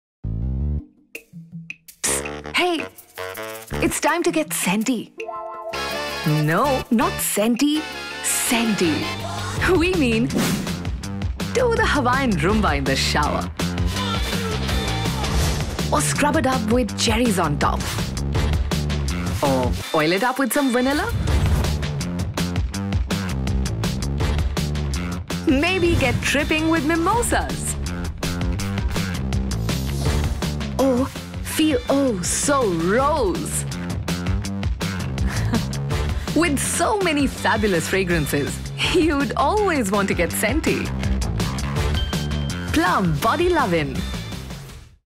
Plum BodyLovin' | GetSCENTY | 10+ FAB Fragrances | Vegan Body Care
We know what you’re thinkin’ folks, but this ain’t no typo! We REALLY want you to get SCENTY and we mean it! 😉
Presentin’ 100% vegan bath & body products in not-so-regular fragrances that you’ll fall in love with at first whiff.

Hawaii, Cherry, Vanilla, Mimosa, Rose and so many more FABULOUS fragrances to choose from—which one will you GetSCENTY with? ✨